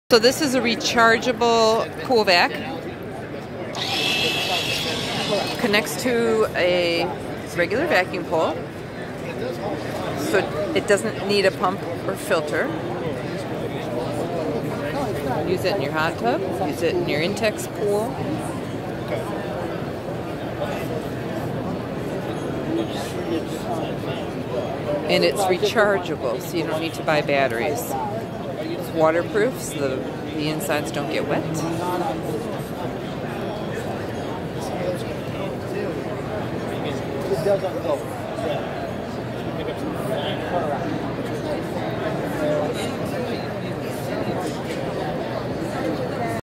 Finally, a vac for Intex Pools!
Yes, now you Can vacuum your Intex Pool or Swimspa with this rechargeable Volt Vac.  Check it out!!